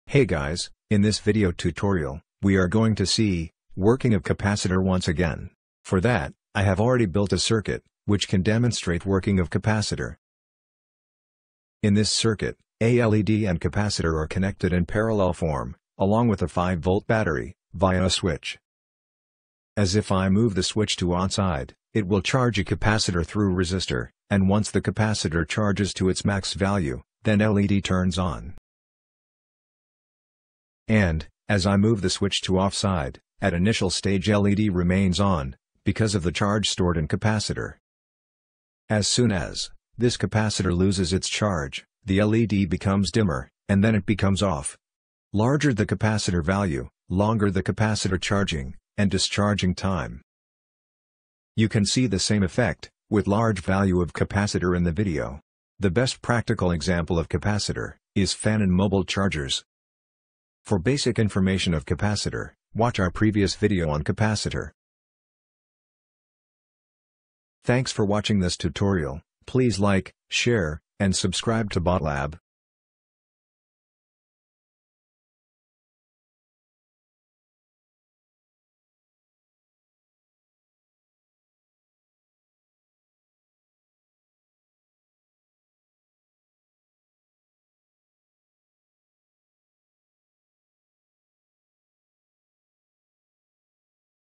Everything about Robotics
This is video # 5 again. Capacitor is a tough topic to understand. So we made a circuit to explain you the stuff.